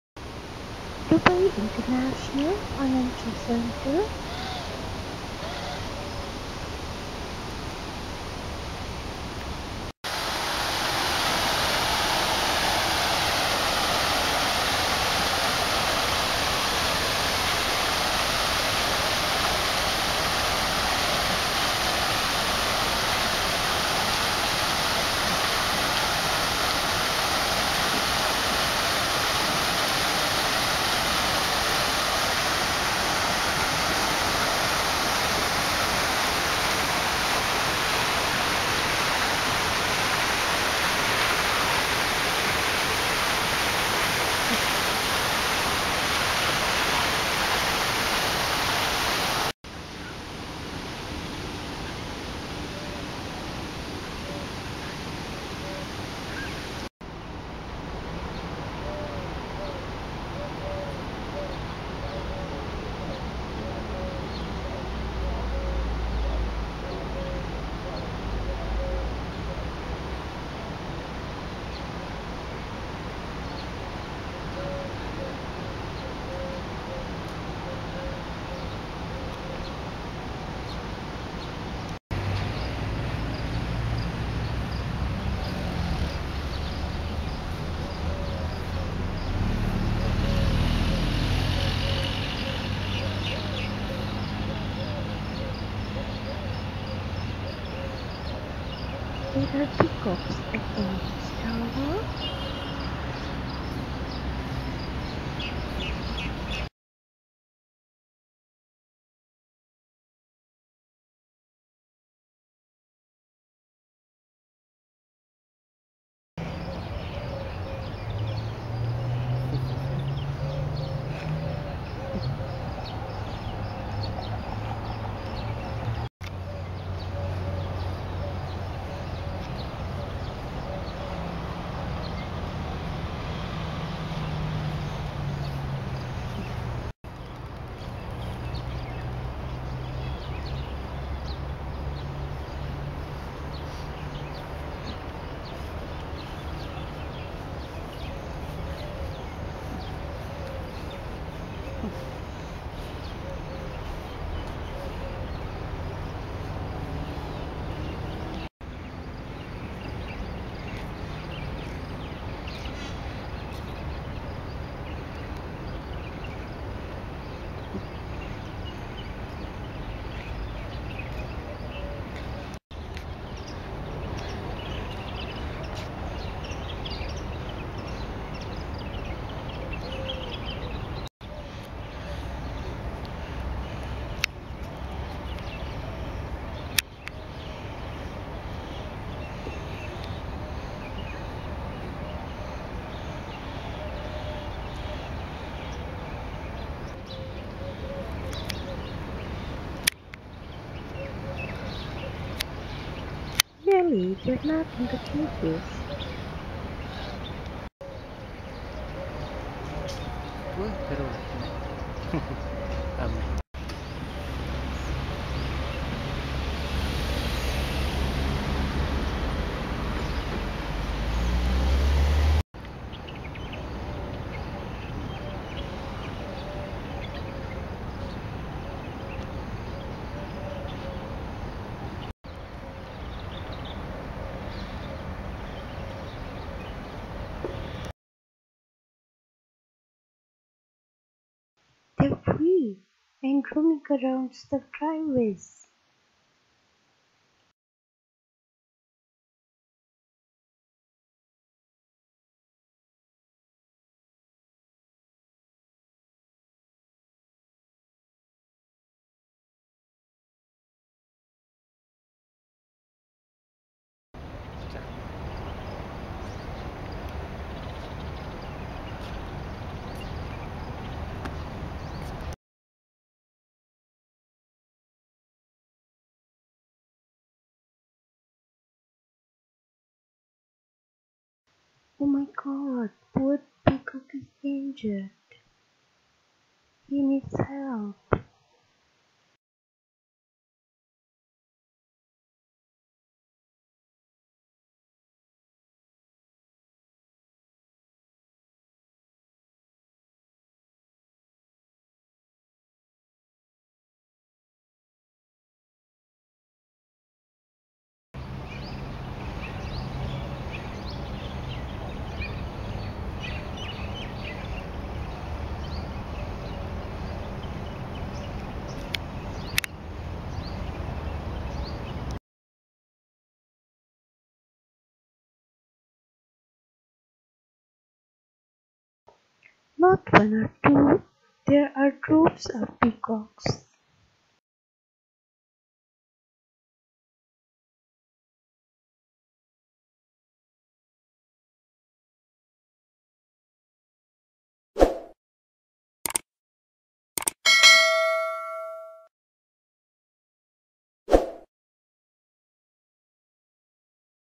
There are PEACOCKS AT THE EMIRATES TOWERS.
Morning walk
Dubai International Financial Cent re (DIFC)
Emirates Towers
Jumeirah Emirates Towers